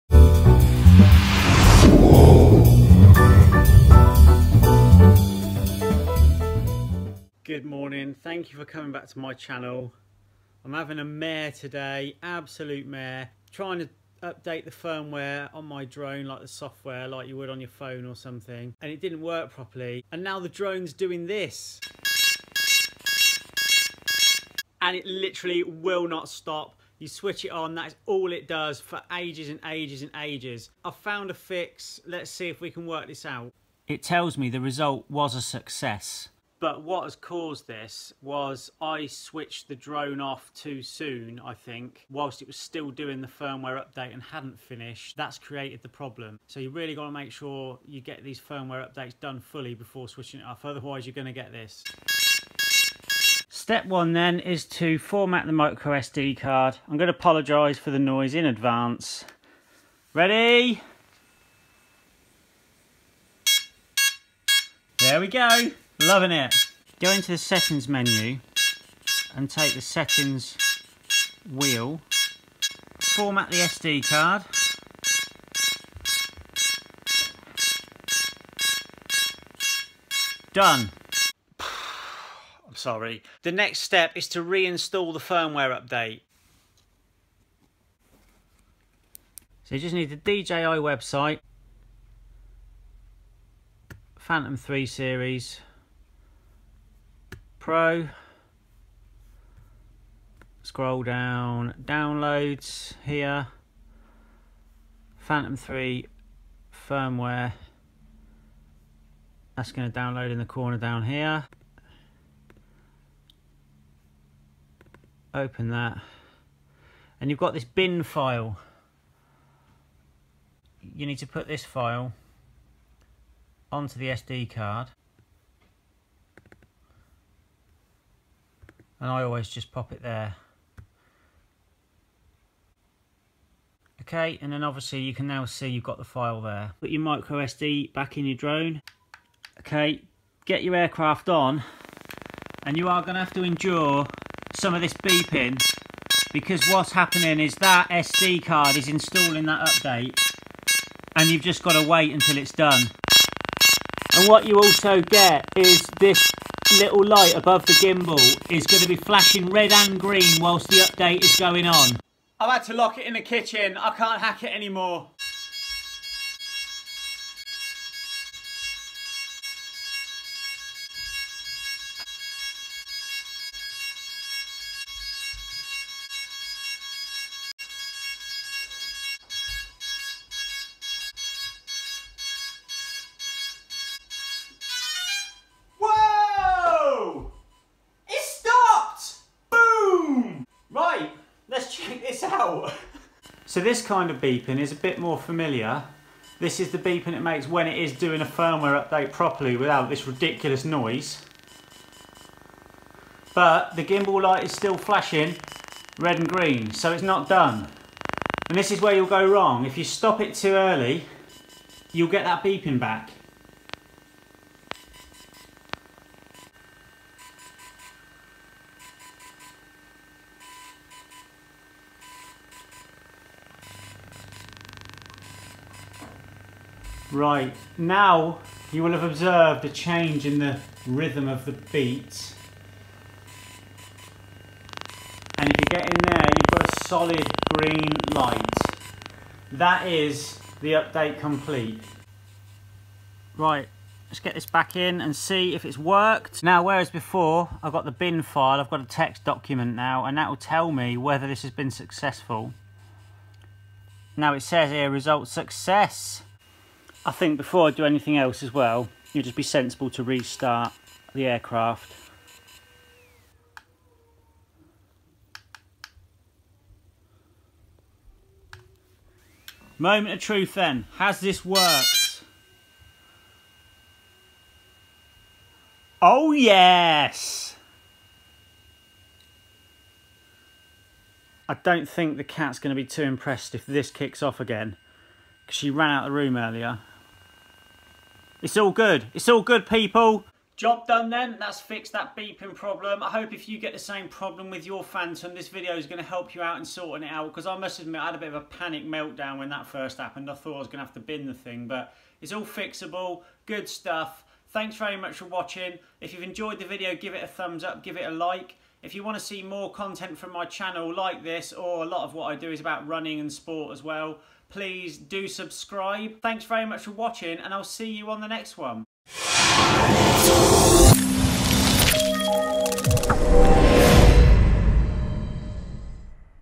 DJI Drone won't stop beeping after Firmware update Phantom 3 Professional
How to stop your drone beeping continuously if a firmware update fails

Cameras:
GoPro Hero 4 Silver

Drone:
DJI Phantom 3 Professional